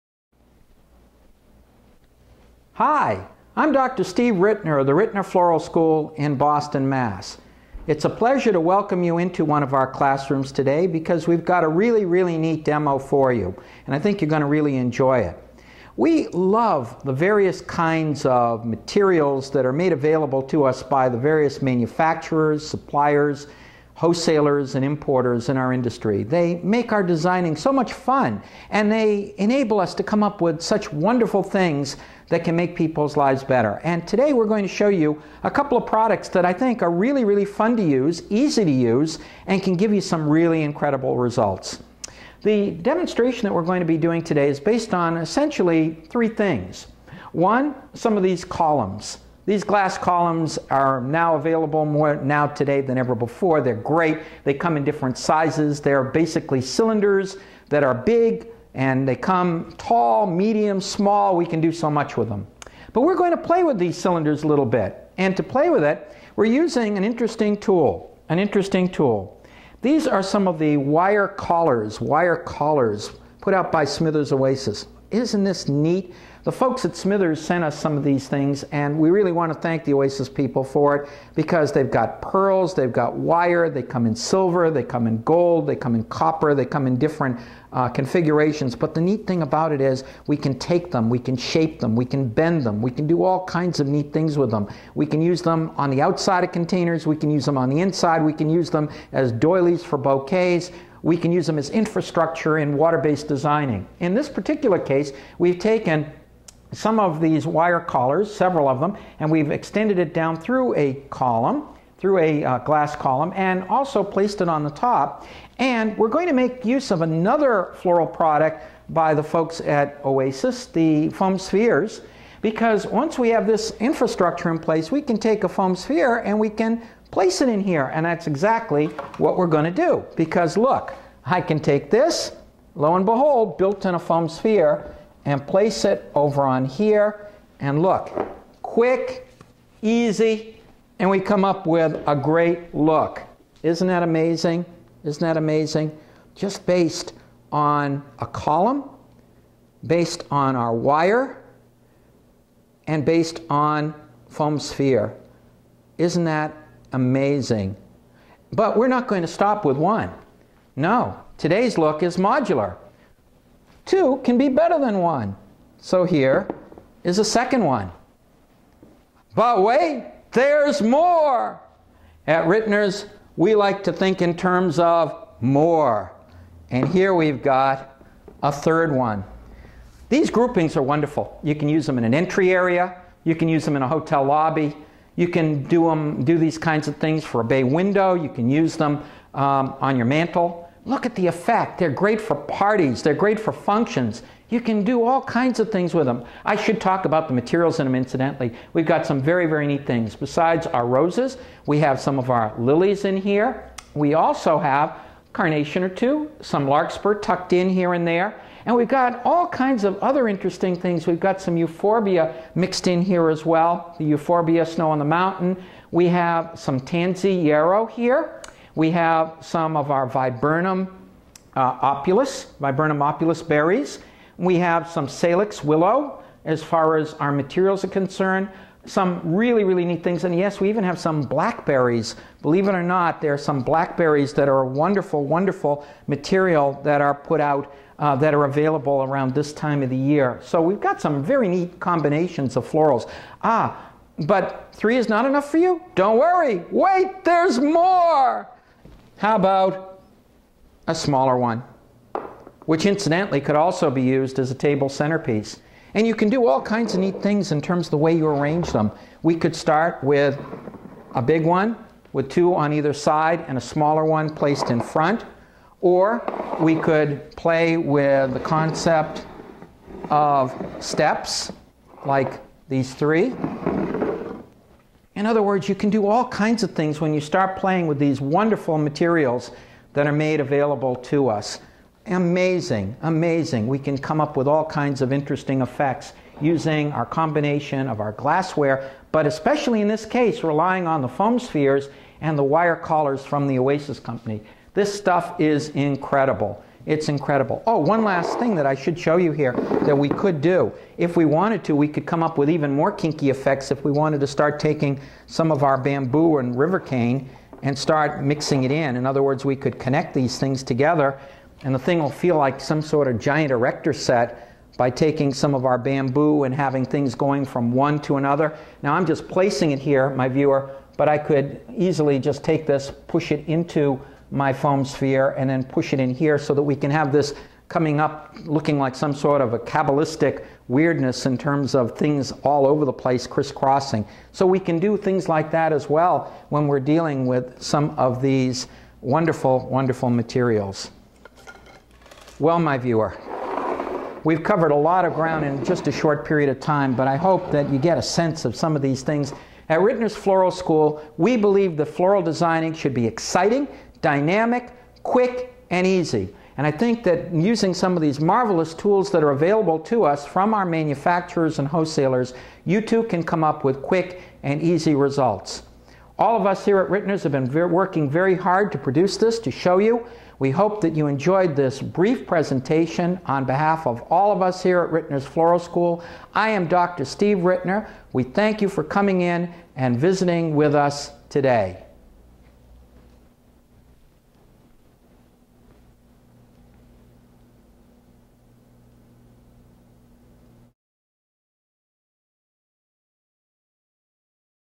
Party Floral Art on Glass Columns With Wire Collars and Foam Spheres
At Rittners Floral School in Boston, MA we love the many wonderful products that our manufacturers, wholesalers and suppliers make available to us. In this neat little presentation we show some wonderful party floral art created on glass columns incorporating wire collars and foam spheres from the Oasis Company. Sit back, relax and enjoy!